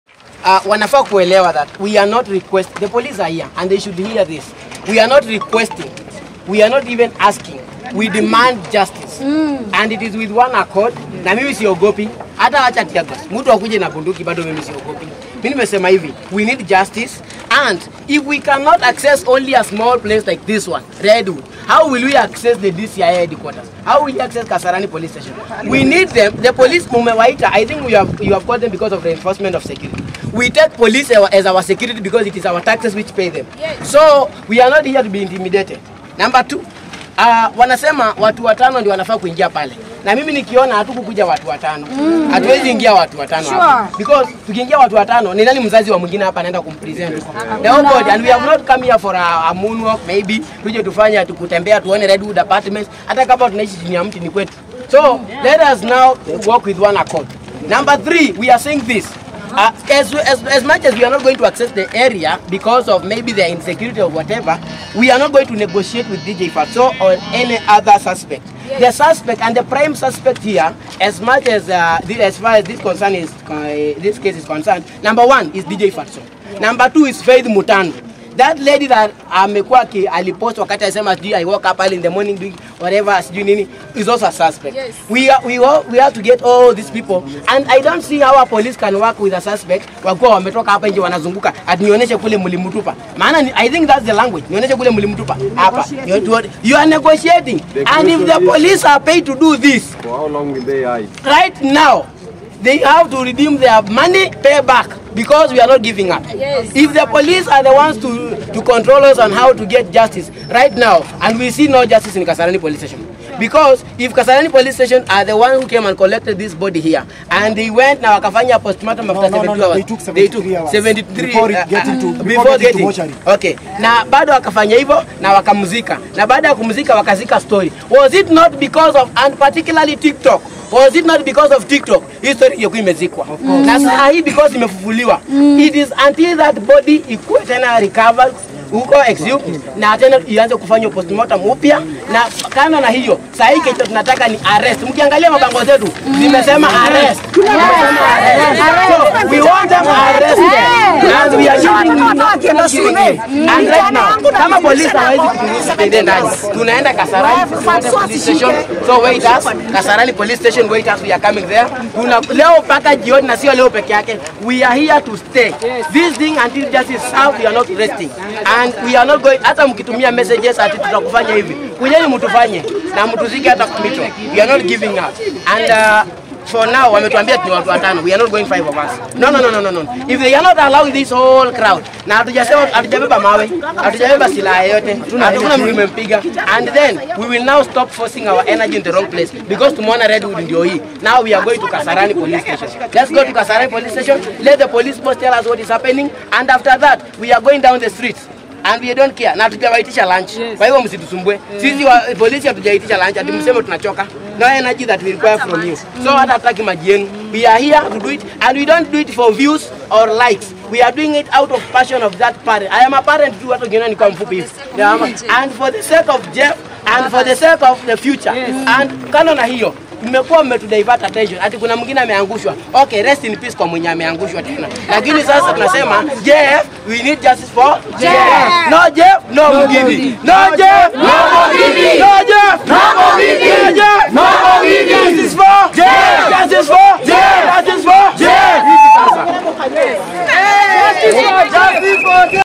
MUINGI KURAKARIO NI DJ FATXO NA KUUGA ARREST HIM
angry Kenyans are bitter because of Jeff death according to them they want  justice to happen soon